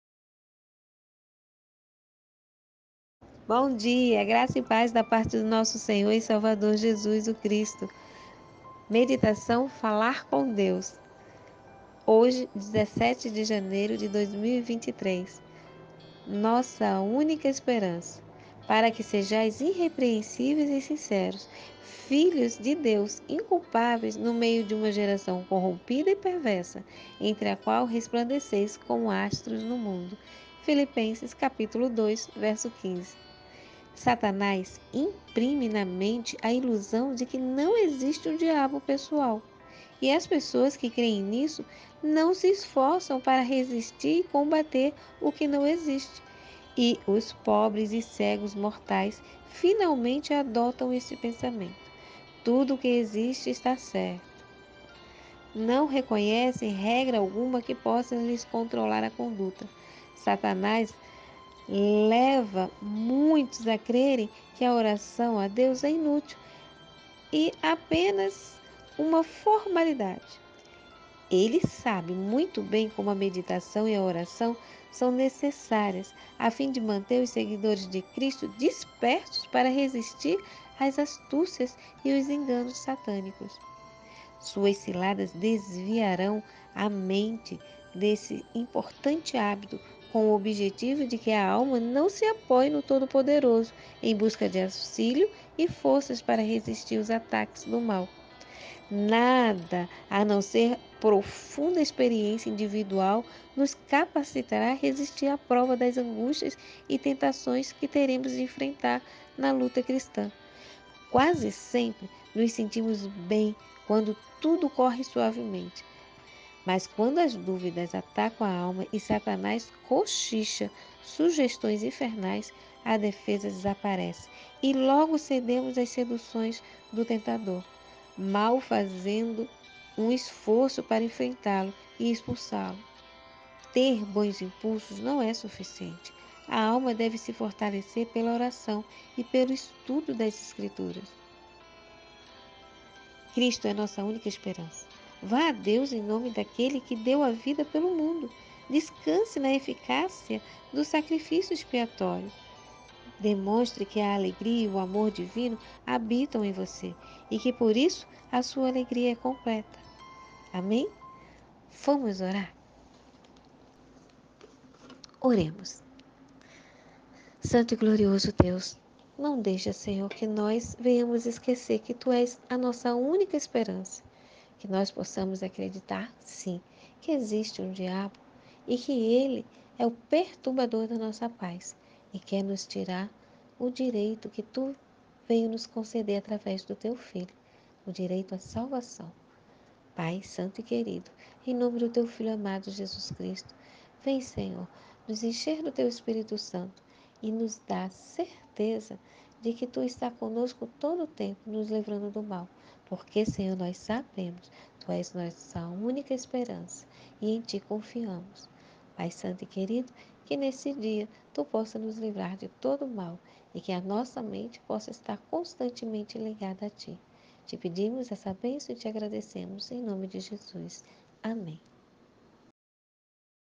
17/01/23 Meditação: Falar com Deus - Nossa Única Esperança
Nossa única esperança é está aos pés de Cristo.Sem Ele nada somos e nada podemos fazer.

Podemos abrir o coração a Deus e contar-lhe todas as nossas aflições e Ele nos diz:
"...Neste mundo vocês terão aflições; contudo, tenham ânimo! Eu venci o mundo". João 16:36

Ministério Flôres da Fé